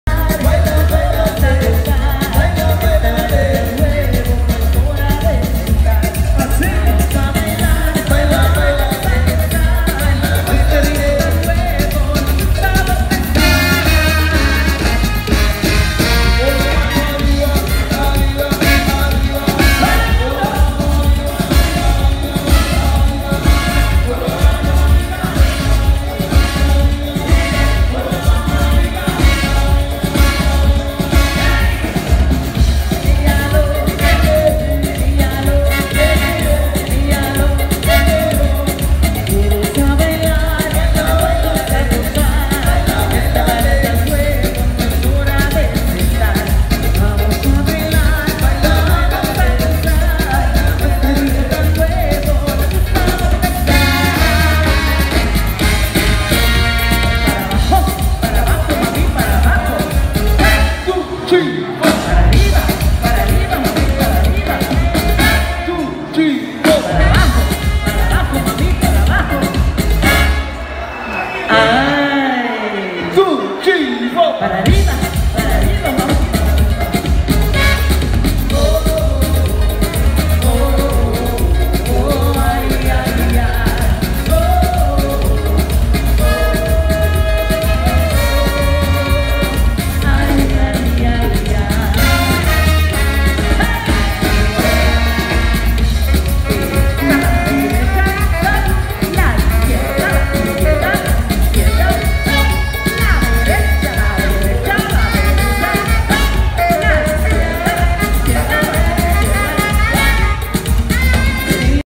Gilharry 7 Band Live in Mexico
“Gracias por la invitación a La Presidenta Municipal “ Its Lit 🔥 🔥🔥❤️God Is Good 🙏🏾Its Feels So Blessed to be Back on Road Again  Performing Live for Audience - We love ❤️ You Mexico 🇲🇽 -
Gilharry 7 Band  Live in Concert 🎶 @ Bellavista Chetumal México